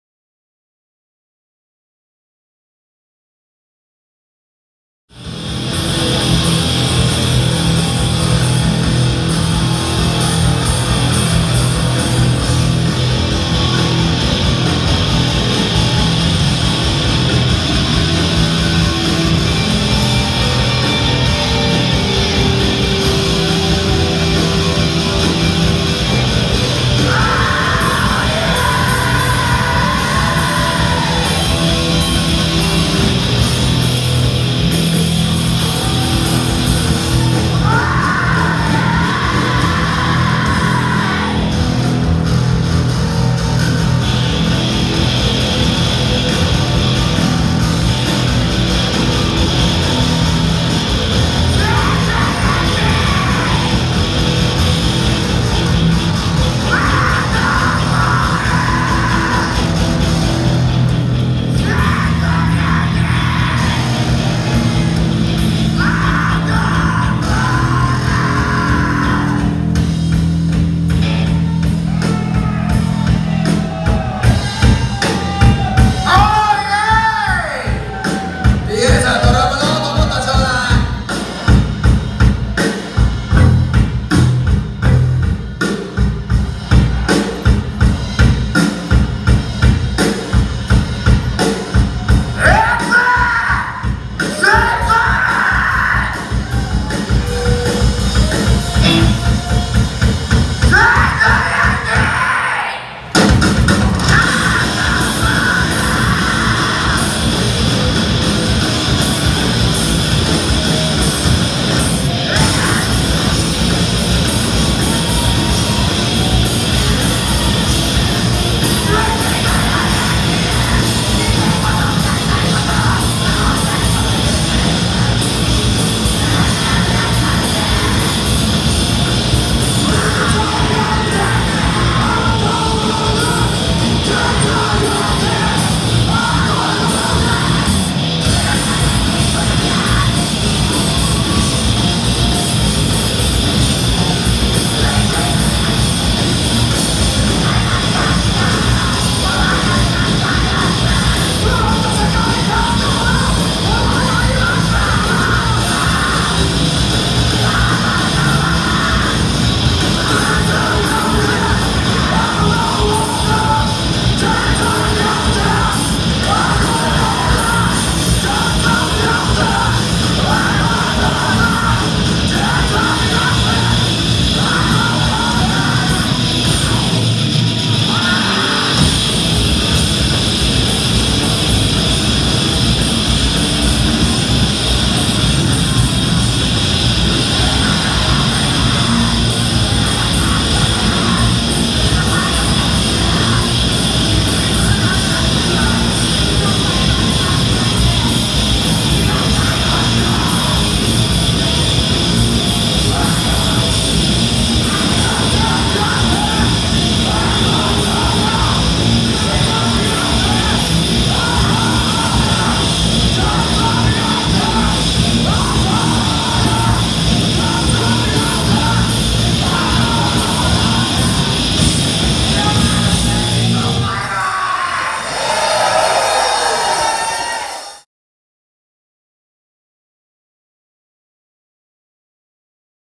WARHEAD 2018/09/23@京都大学西部講堂
THE LIVELY VOICE BLEEDING
FOR LIEF!!!
2018/09/23@京都大学西部講堂

WARHEAD
S.H.I
STRUGGLE FOR PRIDE
ENDON
FEROCIOUS X
QUANG DUC ASYLUM
DJ SHIGACHANG
DJ HOSOKI